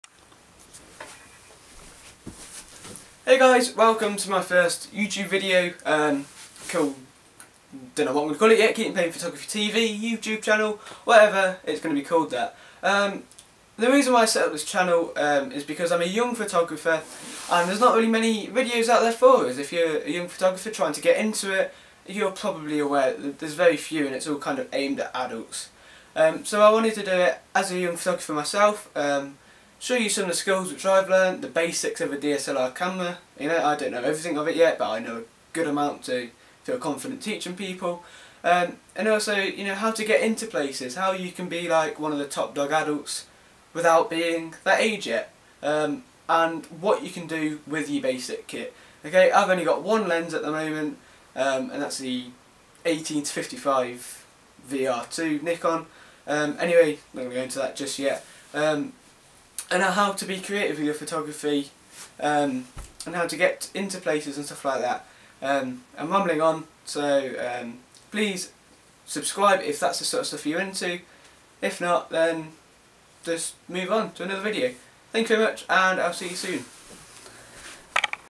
My first video and why I set the channel up!
I know it's not the Greatest quality! But as time goes on things will improve(promise), I created this video to explain to you guys why I started to make Youtube videos.
1. There are very little videos out there for you photographers
2. I wanted to share with you my knowledge to try and help you Improve your photography
3. to show you some tricks and hacks with only the basic kit
Thanks